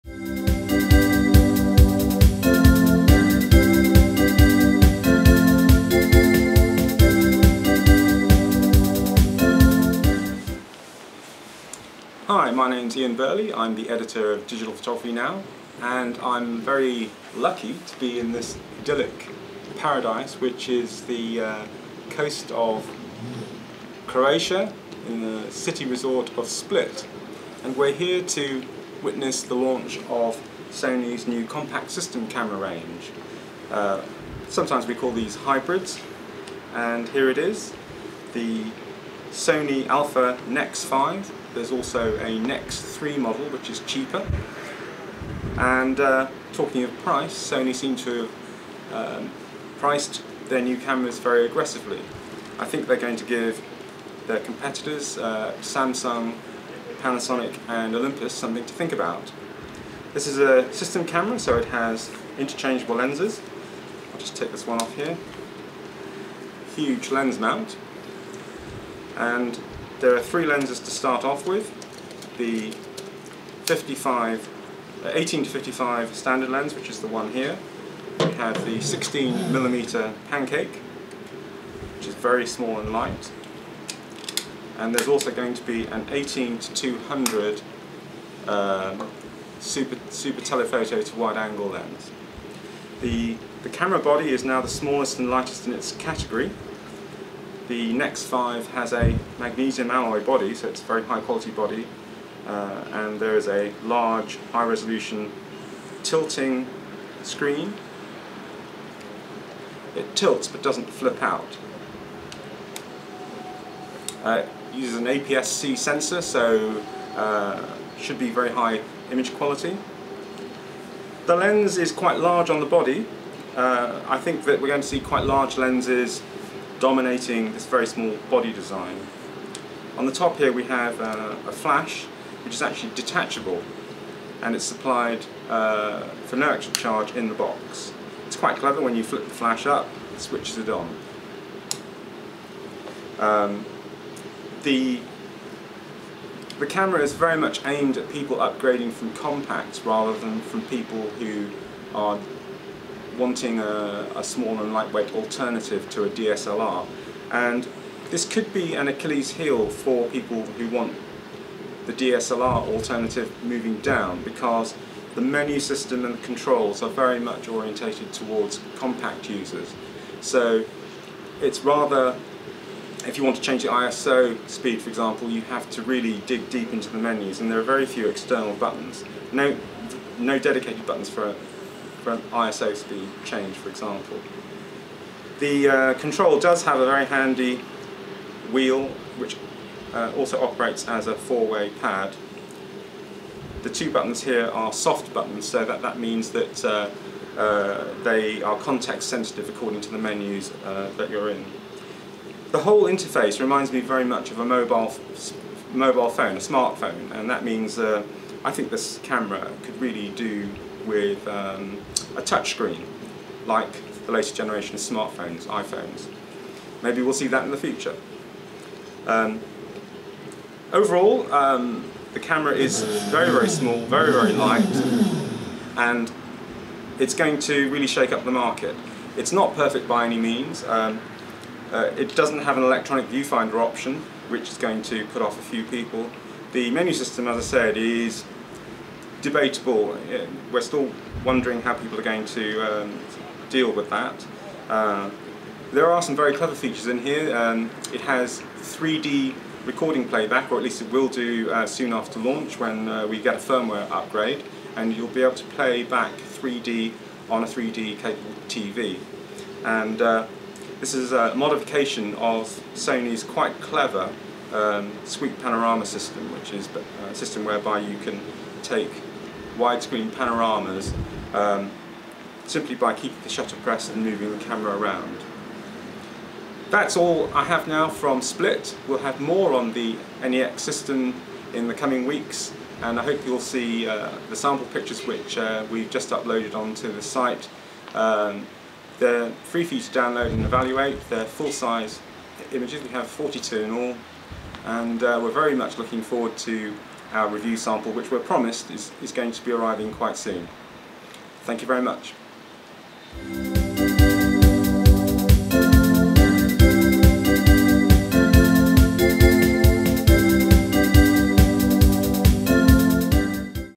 Sony Alpha NEX-5 compact system camera introduction by DPNow.com
Ian Burley, DPNow.com's editor, introduces the Sony Alpha NEX-5 from Split, the Croatian resort city on the Adriatic, where Sony Europe unveiled the NEX system to European journalists on Tuesday, 11th May, 2010.